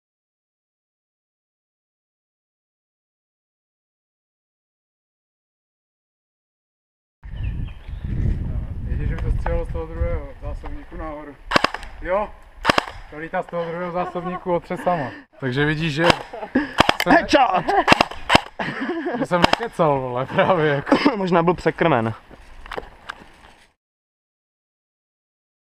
Airsoft Magazine Fail / Cyma MP5 120BBs
Bender si při hře stěžoval, že na něj střílí kule z vlastní zbraně když hraje. Smál sem se mu, že střílí do stromu před sebou. Tak sme pak rozlouskli tu záhadu :)